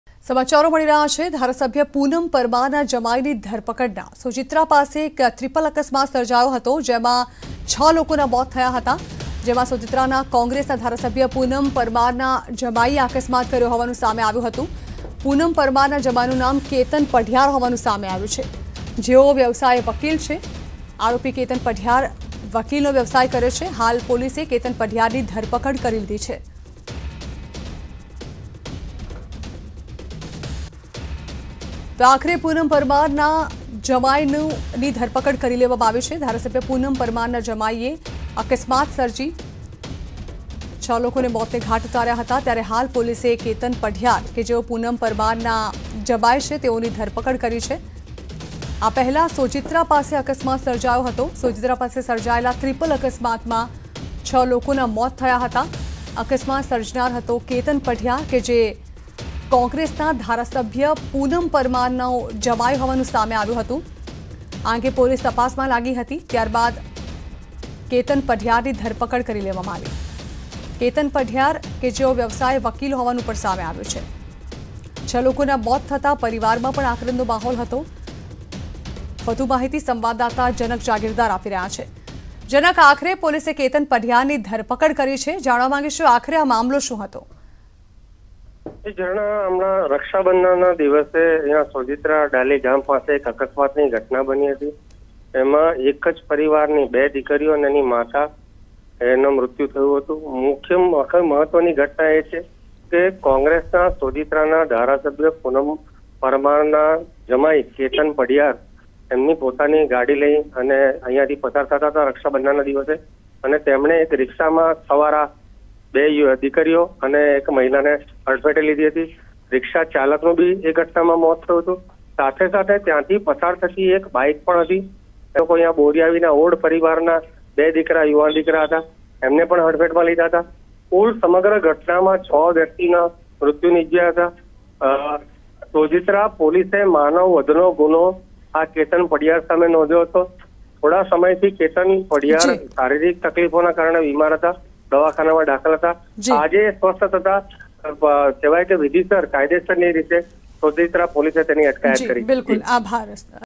Breaking News : અકસ્માત કરનાર MLA નો જમાઈ ઝડપાયો | Sojitra Accident | Gujarati News|News18 Gujarati
Son-in-law of MLA who caused an accident in Sojitra arrested. Son-in-law of MLA Poonam Parmar arrested.

Sojitra માં અકસ્માત કરનાર MLA નો જમાઈ ઝડપાયો.MLA Poonam Parmar ના જમાઈની કરાઈ ધરપકડ.